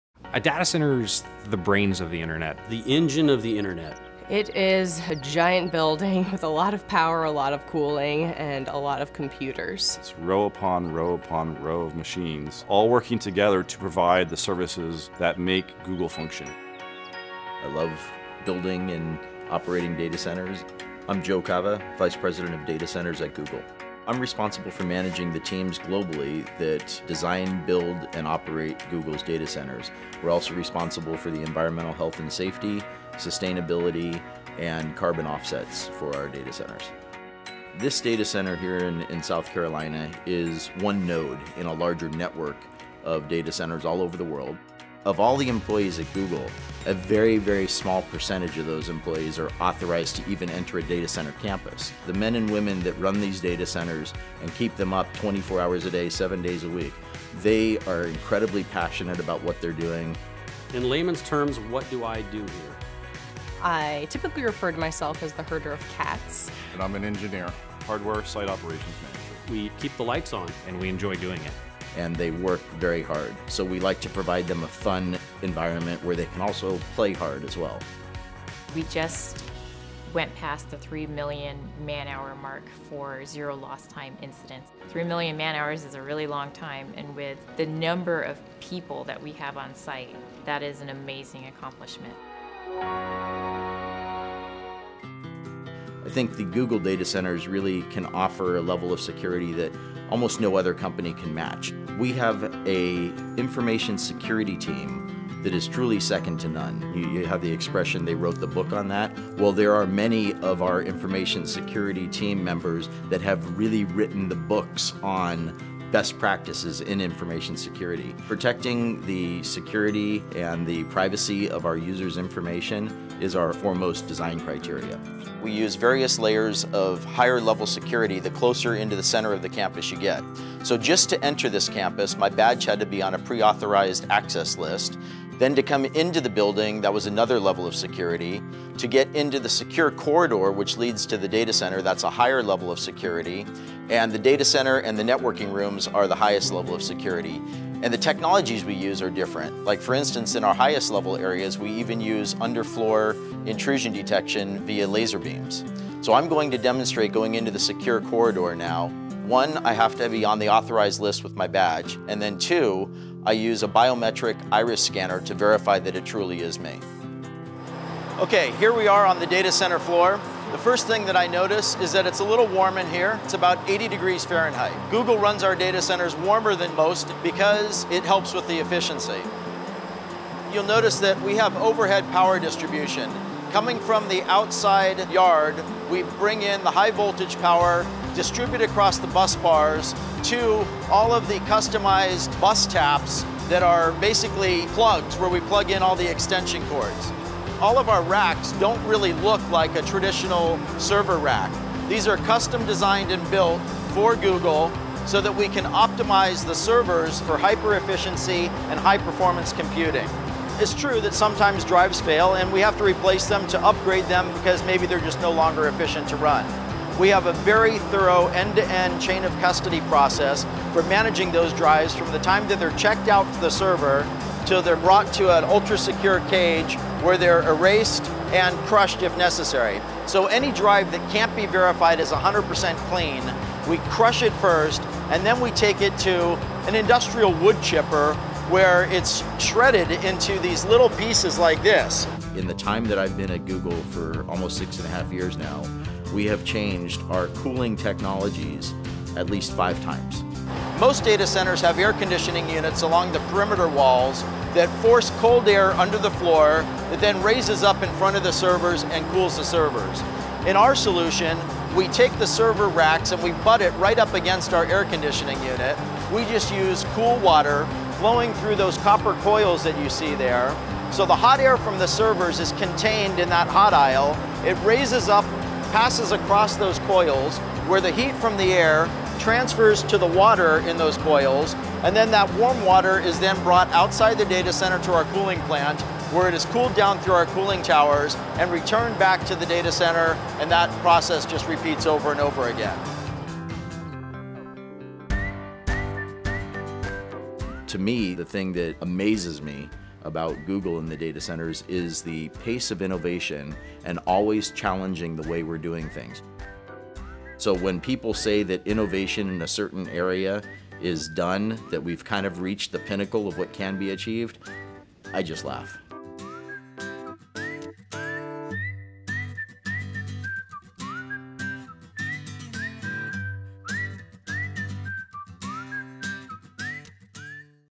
Inside a Google Data Centre - Hand's On Tour!
One of Google's top Data Center VP's, Joe Kava gives a tour inside one of the World's largest data centre owners - Google!

Covering areas surrounding security, sustainability and the core architecture of their infrastructure.